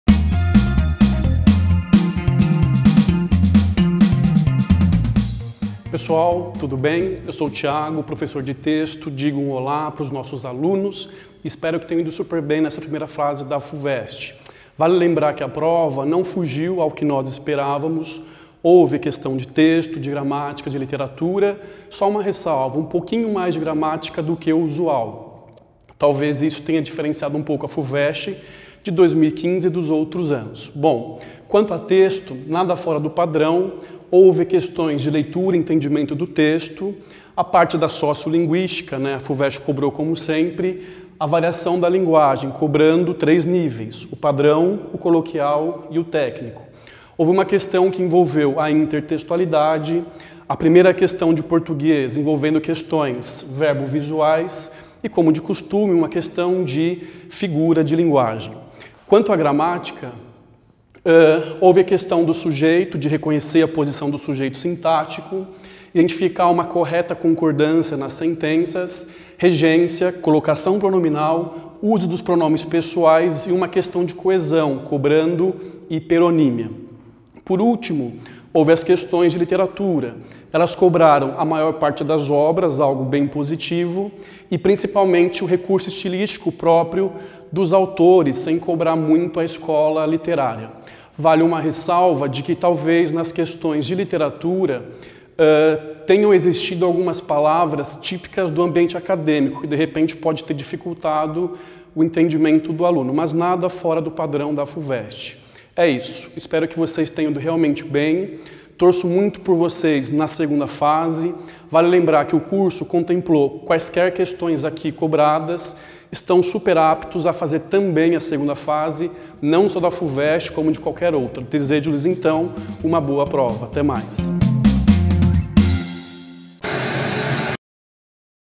Fuvest - 1ª Fase - Português
Professor do Poliedro comenta a prova de Português aplicada na 1ª fase do vestibular 2015 da Fuvest.